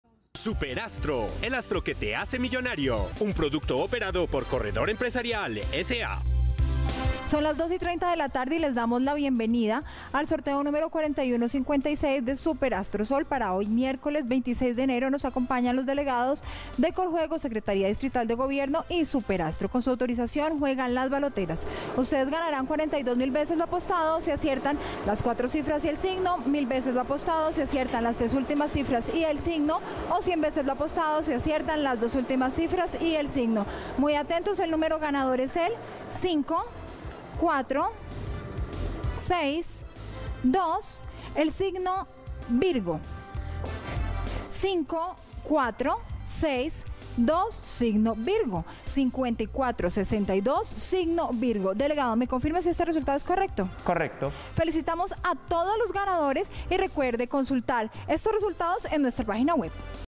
ASTRO SOL Resultado ASTRO SOL del Miércoles 26 de enero de 2022 ✅🥇🔥💰
Hola buenas tardes, te presentamos los resultados de las loterías y chances en Colombia, vamos con el Resultados del Chance SUPER ASTRO SOL del Miércoles 26 de enero de 2022.